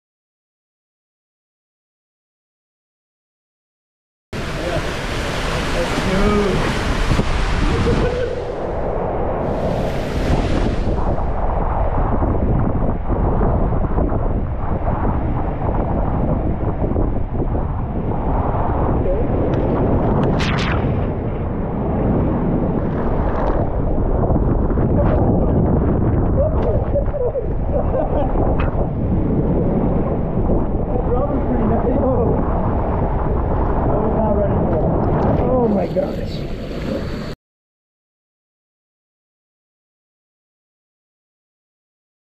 Cyclone Rush Family Slide Baha Bay Nassau Bahamas
Enjoy this POV of the Cyclone Rush Family Slide at Baha Bay in Nassau Bahamas